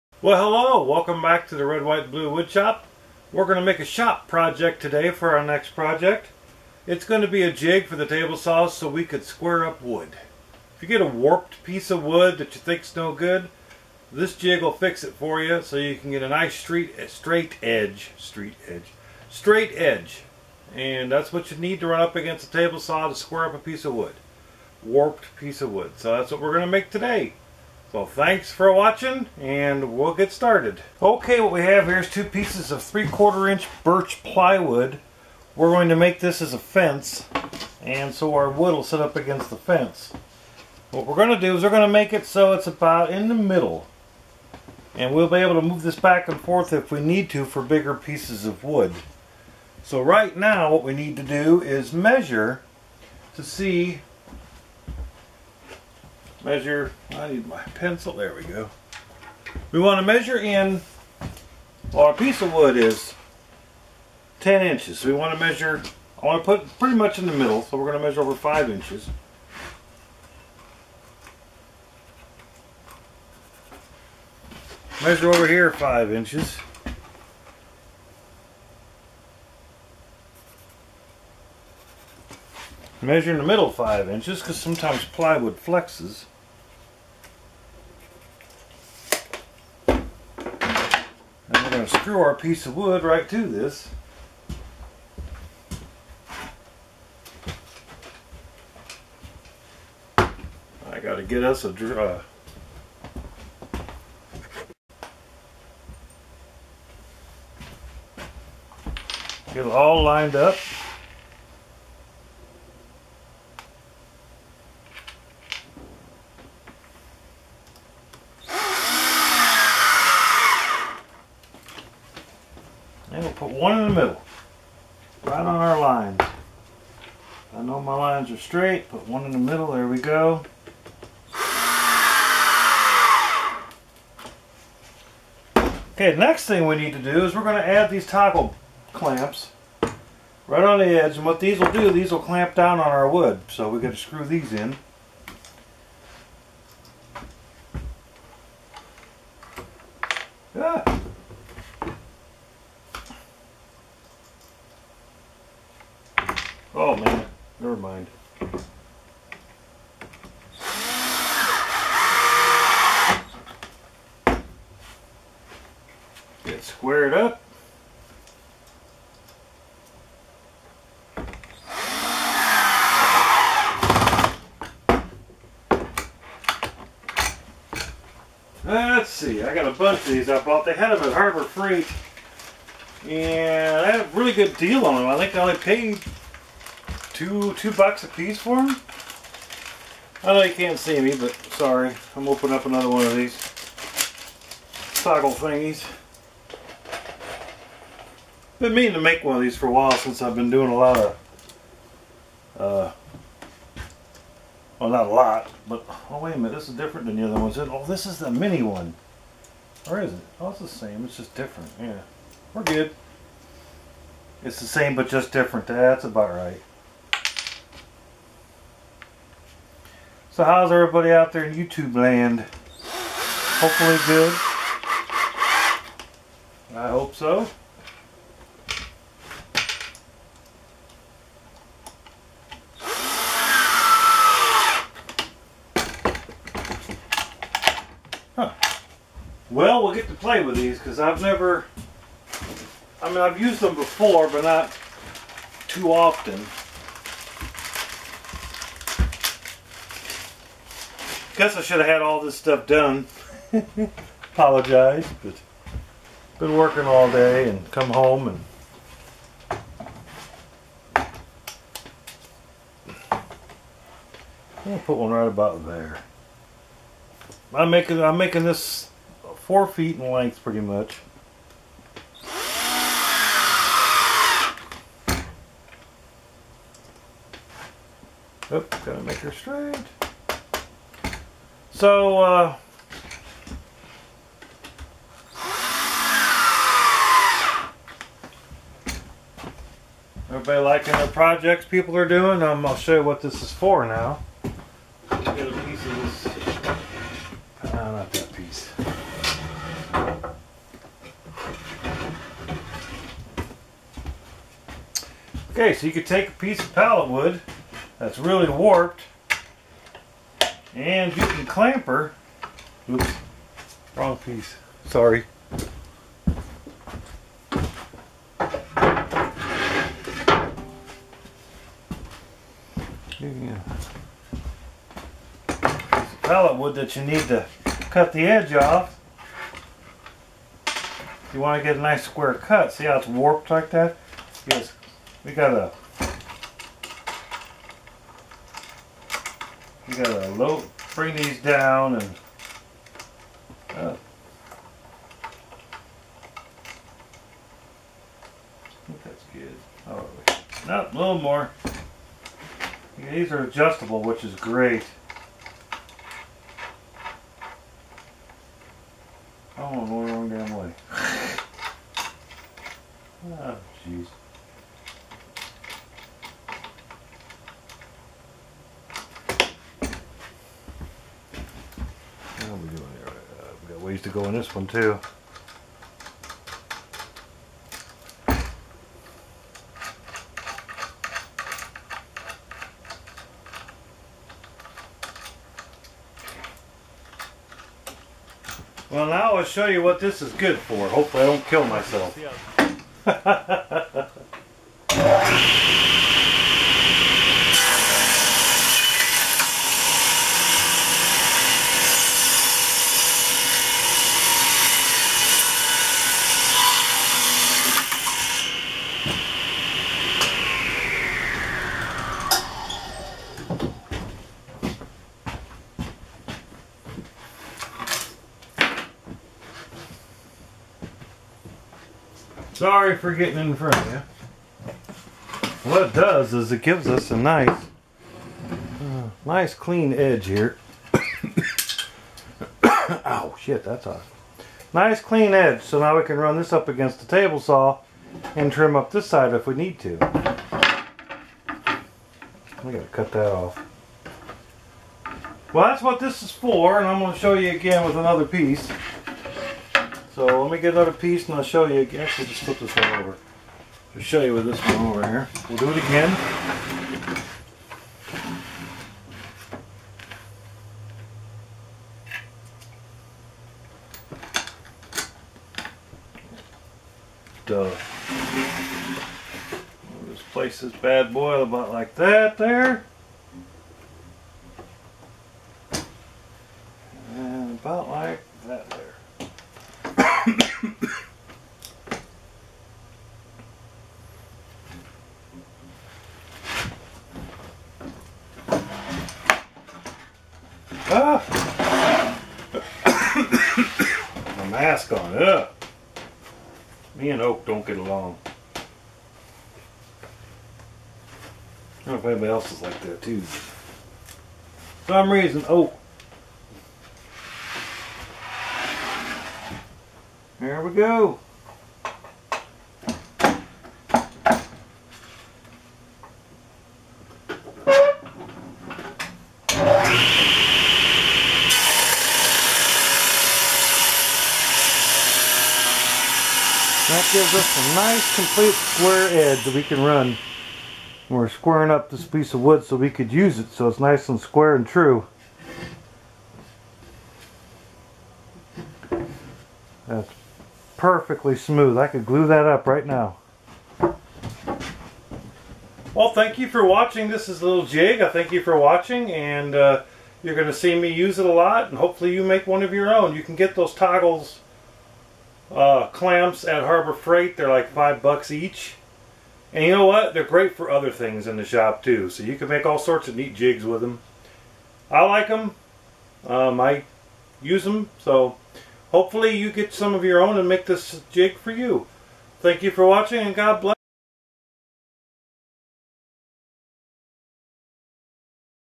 Making a table saw jig to square up pallet wood
One man's journey to learn new woodworking skills while also loosing weight.